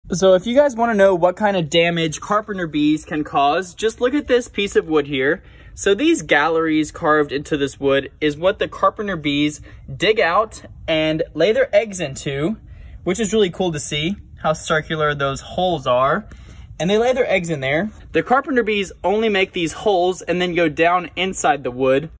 Carpenter Bee Damage
The small holes these carpenter bees make are only the beginning of the damage. The tunnels can extend many inches into he wood.